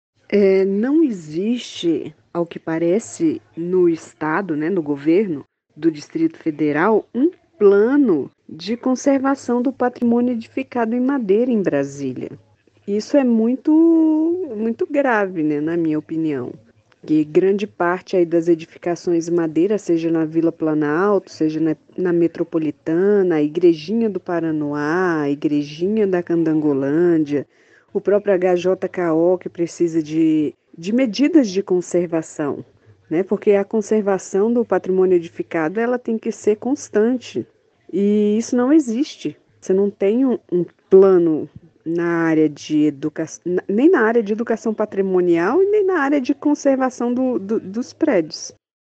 Radialista critica conservação do patrimônio edificado de Brasília
Este vídeo é parte do material especial, publicado pela Agência Senado, sobre os 60 anos de Brasília.

A radialista, escritora e ativista Leiliane Rebouças, filha de pioneiros, faz críticas à conservação das casas e edifícios erguidos junto com a capital. "A conservação do patrimônio edificado tem que ser constante. Não há um plano nem na área de educação patrimonial nem na área de conservação dos prédios. E isso faz com que a gente vá perdendo aqueles prédios significativos da época da construção de Brasília", afirma Leiliane.